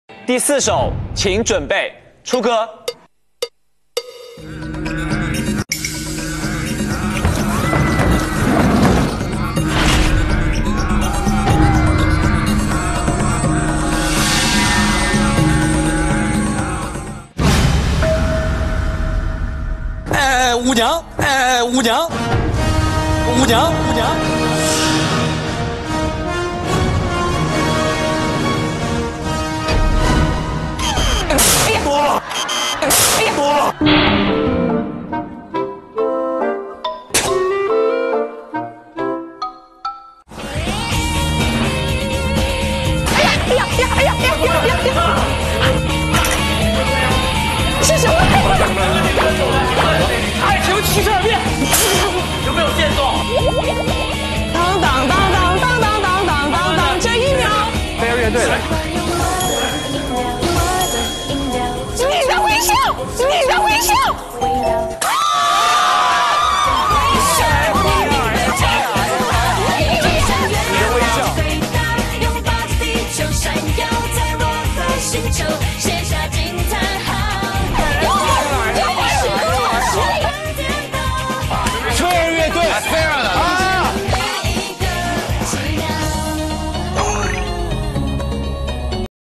猜歌技能大比拼，歌名就在嘴边众人却一片空白，吴莫愁打破得分荒
猜歌技能大比拼，歌名就在嘴边众人却一片空白，吴莫愁打破得分荒,本视频由浙江卫视综艺咖原创提供,19074次播放,好看视频是由百度团队打造的集内涵和颜值于一身的专业短视频聚合平台